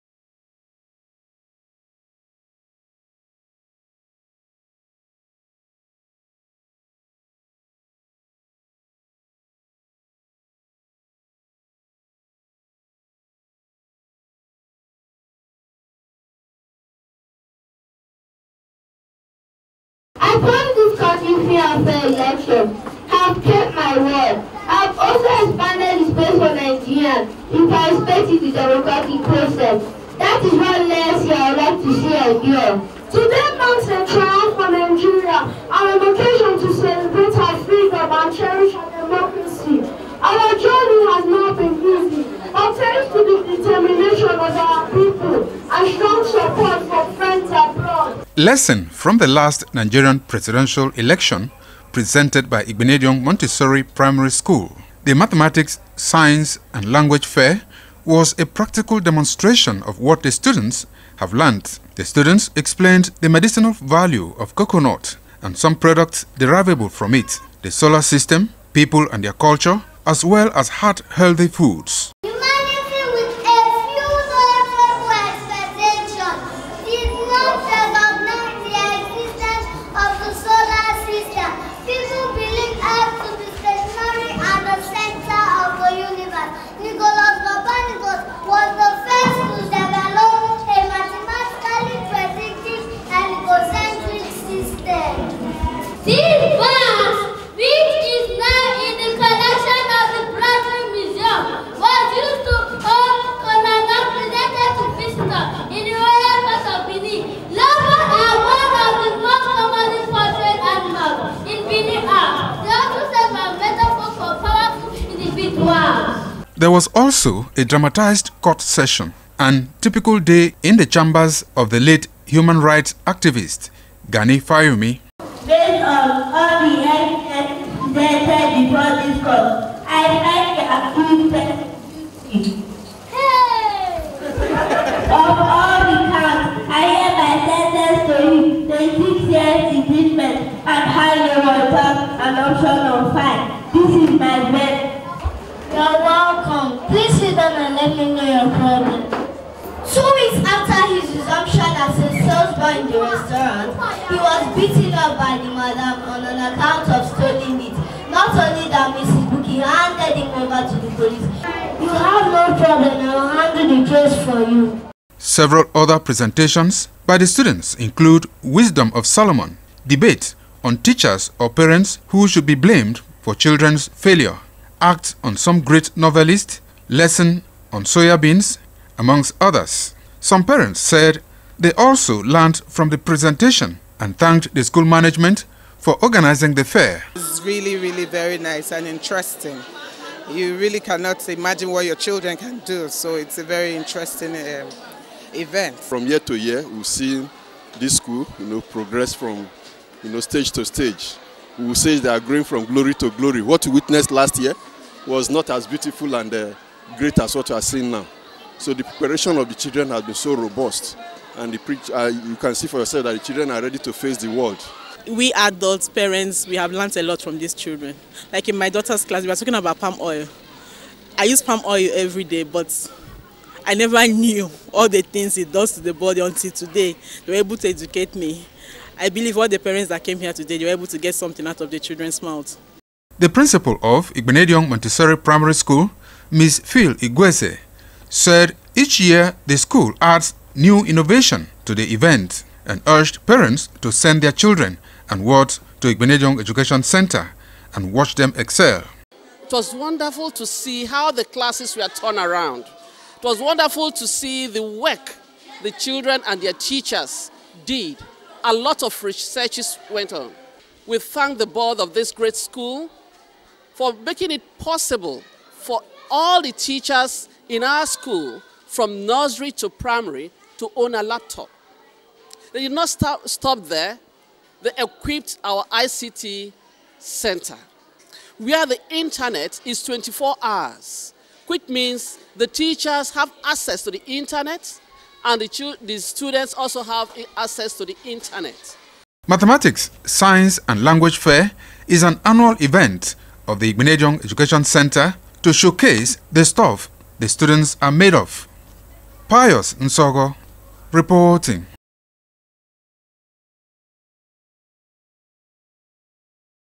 I.E.C Montessori Primary School Holds 2015 Maths, Science And Language Fair
The annual Mathematics, Science and Language Fair of Igbinedion Montessori Primary School, Benin City has been held.